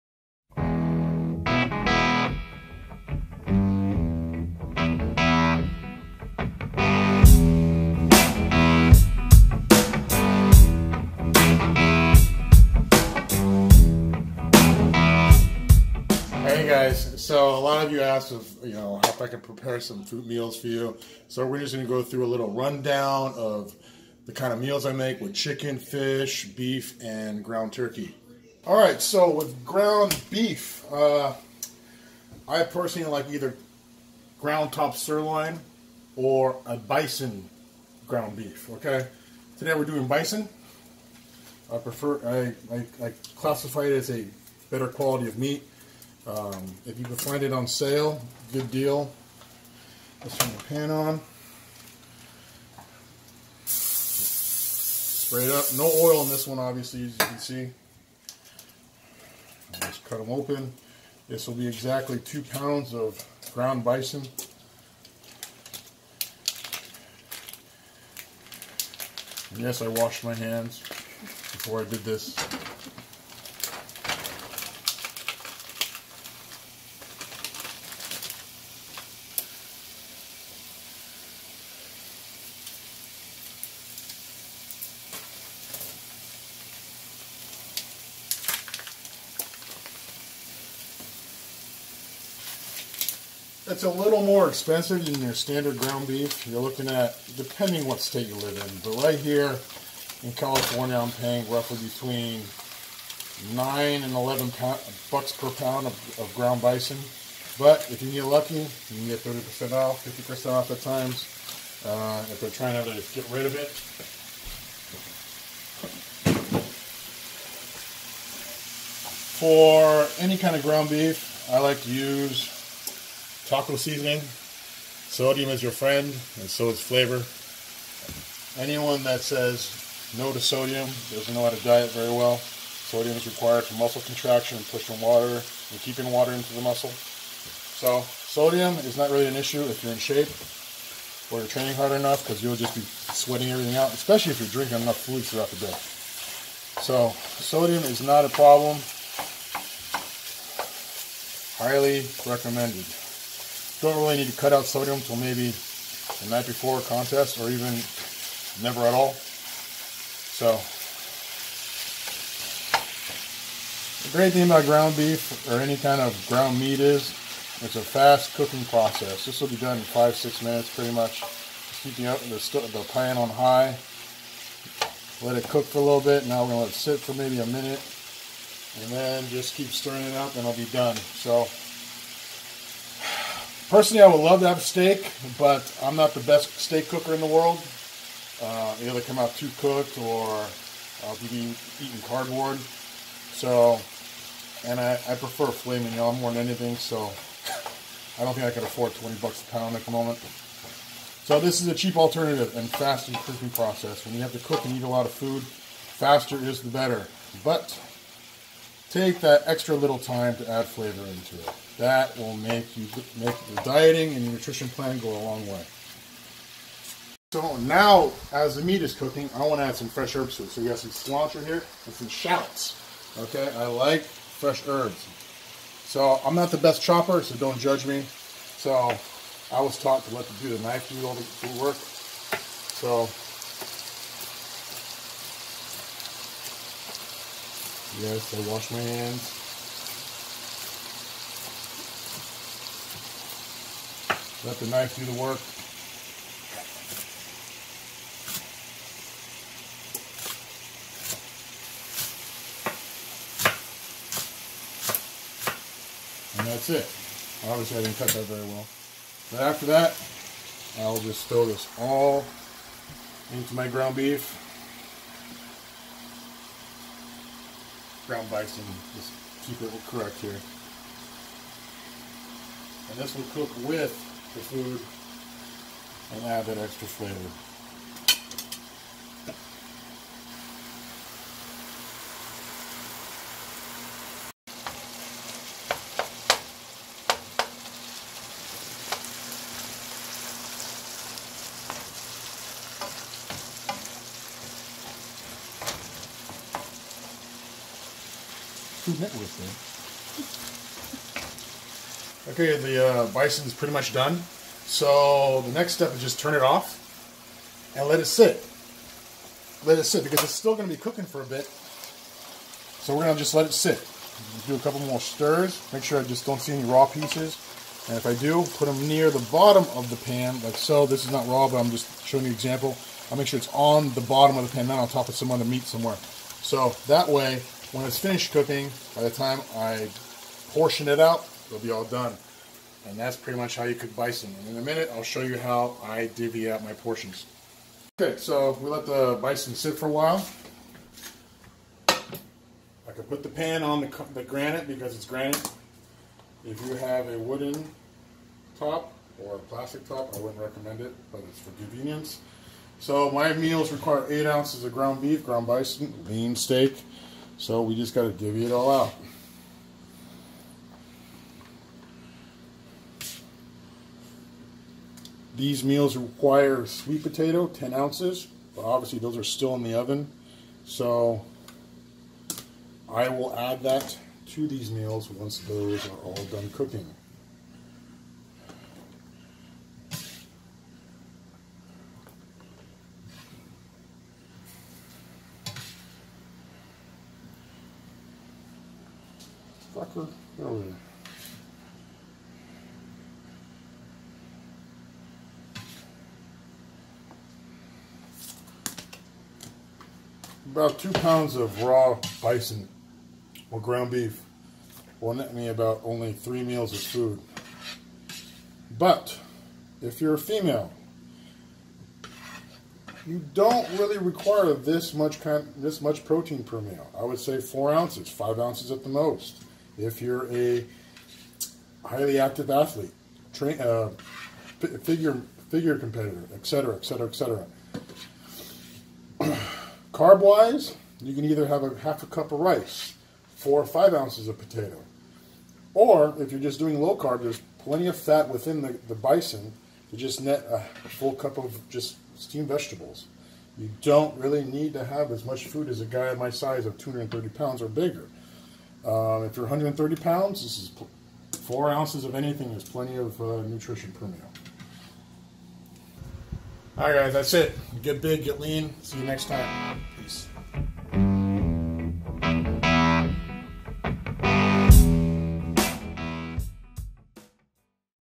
EATTOBUILD SERIES: Ground Bison/Lean Ground Beef
Bodybuilder Marek Piecyk cooks some ground bison